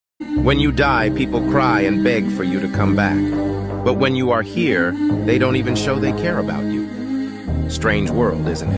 tribute to Herbert wigwe
friends and well wishers pays tribute to Herbert wigwe